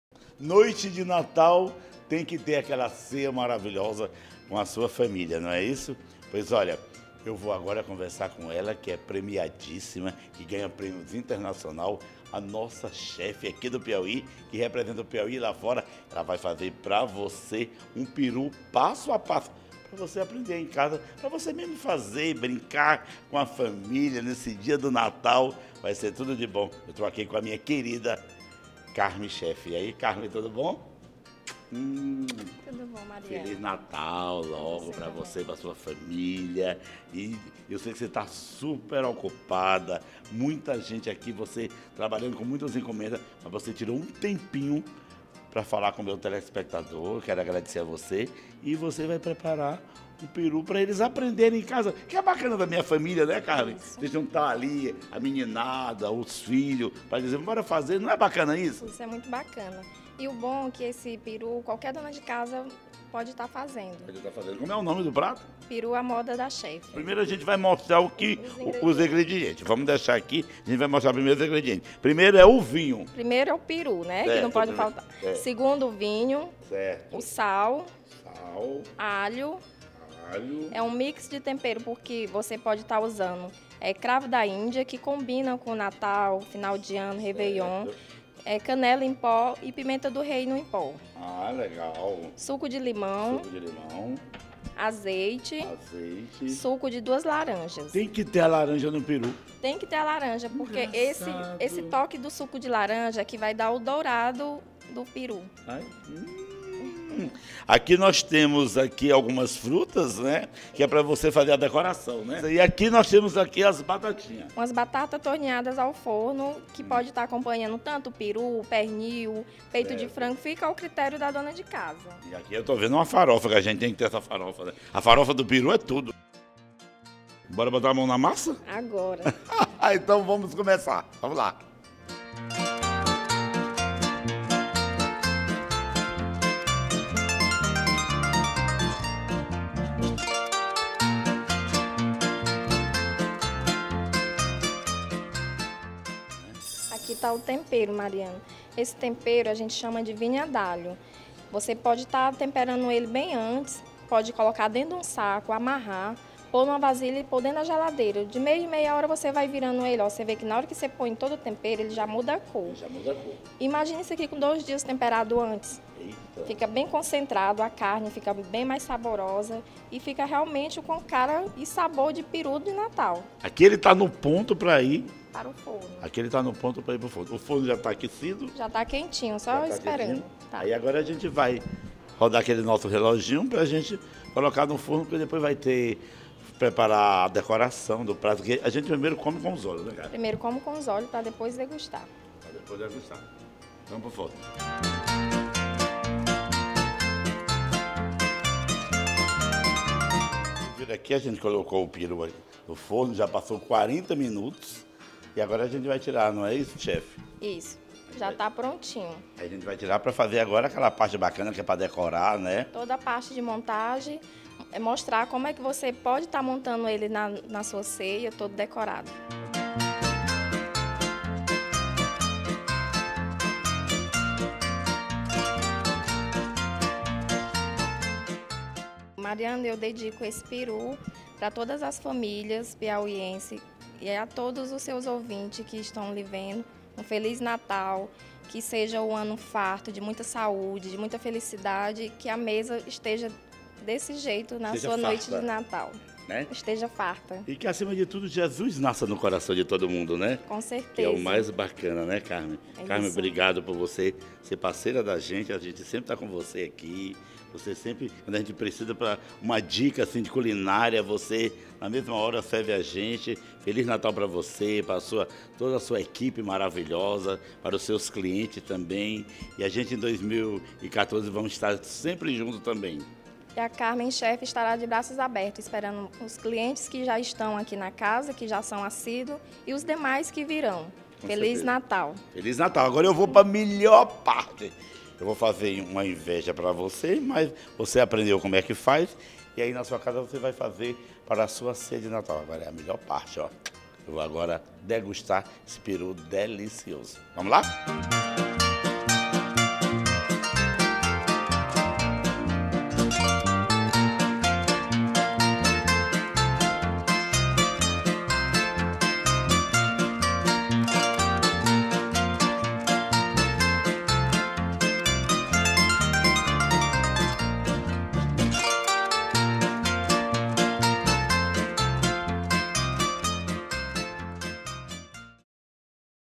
APRENDA A FAZER UM PERÚ DE NATAL
Aprenda com a Carmem Cheff como fazer um perú delicioso para festejar o natal com a família.